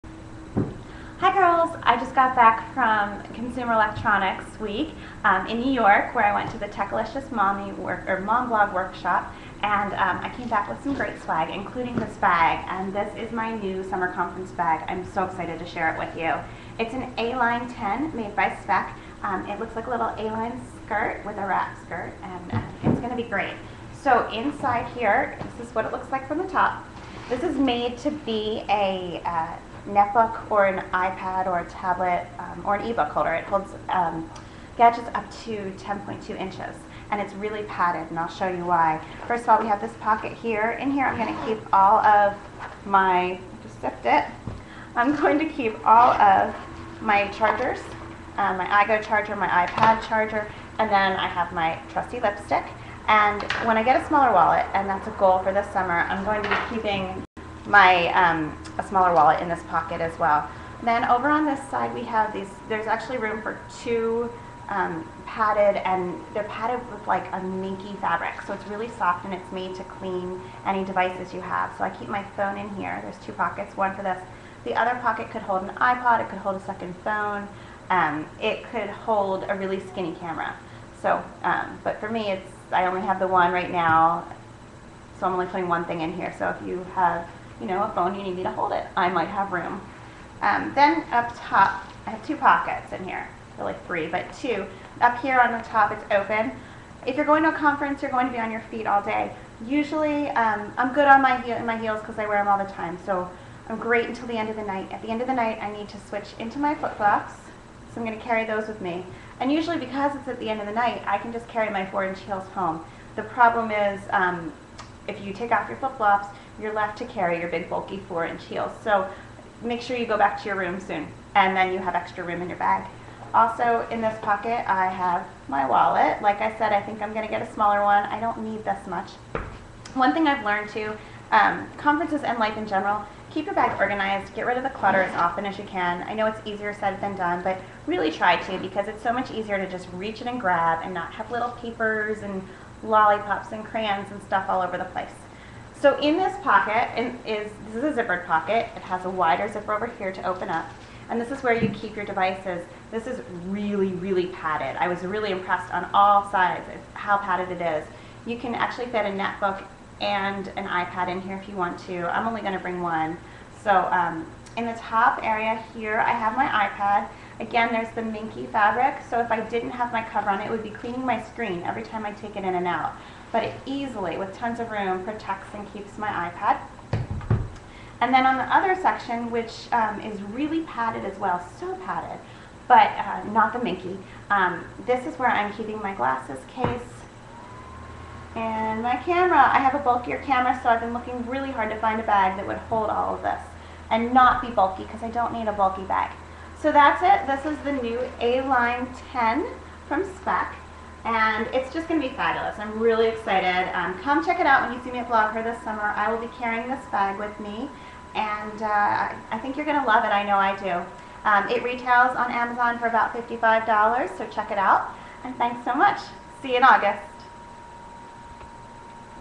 Laptop Bag for iPad or Netbook
The Speck A Line 10 is perfect for blog conferences like Blogher, Blogalicious, Blissdom, Type A Parent and Mom 2.0. Julie Meyers Pron of Just Precious shows and tells as she packs her bag for Blogher.

Speck A-Line Bag for Netbooks and iPad, available at Amazon